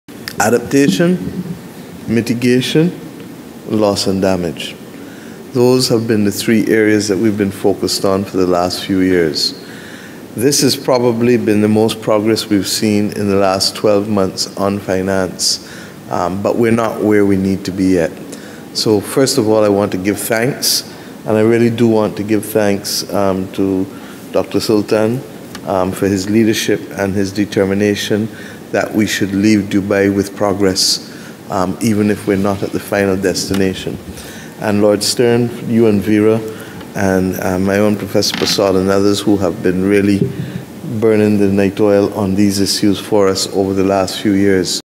Mia Mottey, Prime Minister of Barbados at COP28
"I want to give thanks to Dr. Sultan for his leadership and determination that we should leave Dubai with progress" - H.E. Mia Mottley, Prime Minister of Barbados